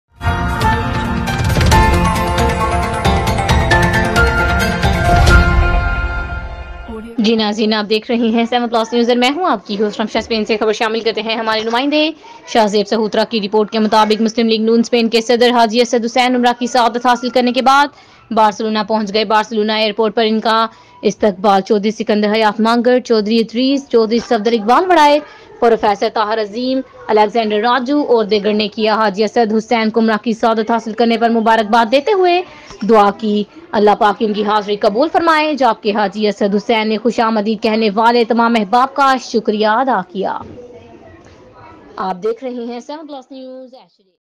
نیوز سپین خبر شامل کرتے ہیں سپین سے ہمارے نمائندے شاہزیب سہوترہ کی رپورٹ کے مطابق مسلم لیگ
Video from JS